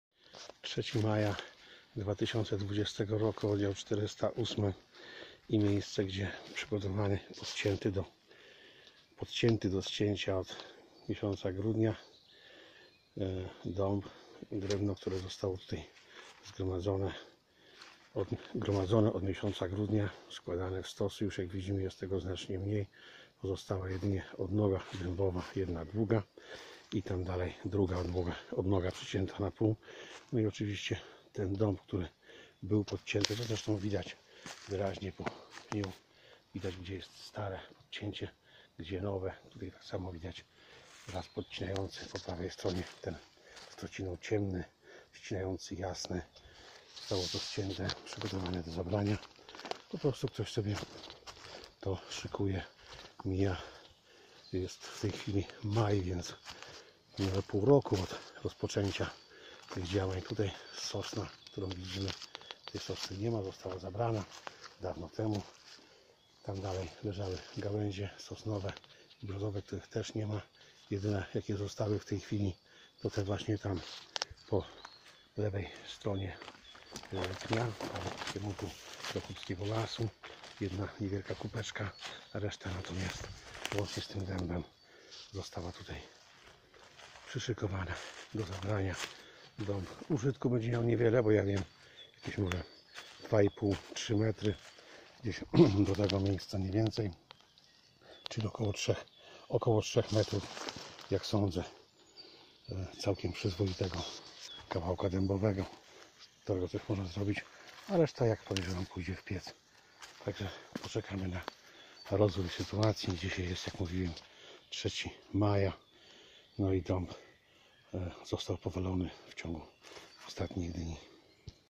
Draństwo leśne .Odc.224.
Oddz. 408.b.Dotychczas trwa niczym nie zmącone  handlowanie lasem , to przedostatni  film  dotyczący  dęba w oddz. 408  gdzie  podobne zdarzenia miały miejsce  dość często.W biały dzień , przez okres wielu miesięcy z lasu wywożone jest drewno,  a jego pozyskanie  wygląda na całkiem legalne.Na żadnej ze stert ułożonego drewna nie było nigdy numeru ,ani cechówki  gdyby były zostały by przeze mnie zauważone a to wystarczający dowód  że drewno zabrano nielegalnie, i wywieziono przez rów oddzielający lasy prywatne od państwowego.